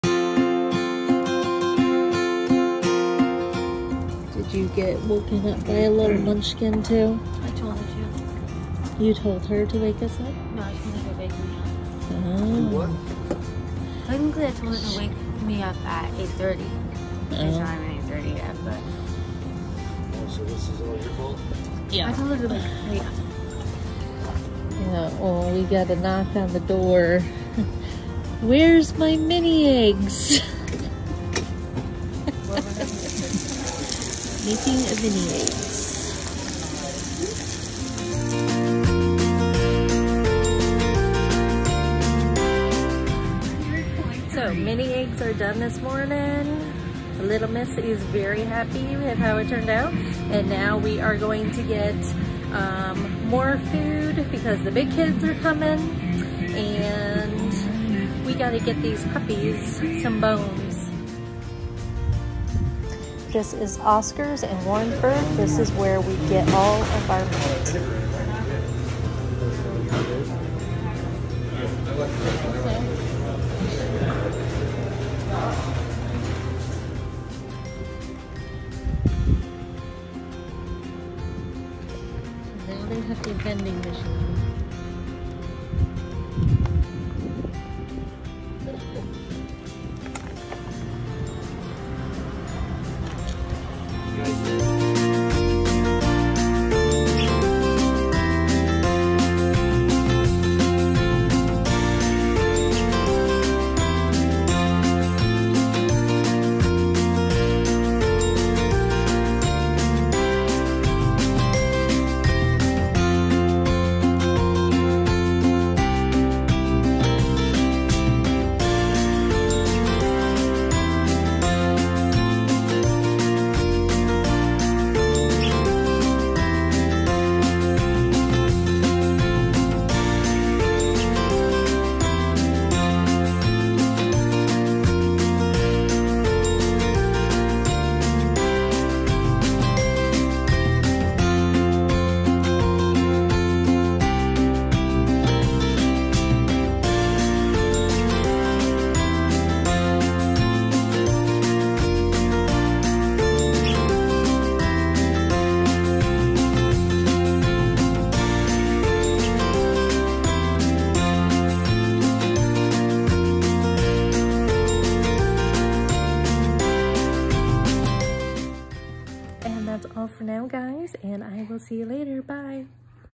Camp food, family and Oscars got a vending machine!
Last camping trip of the year. Camp food as always was awesome, great time spending with family and yup you heard it right Oscars got a vending machine! Crazy!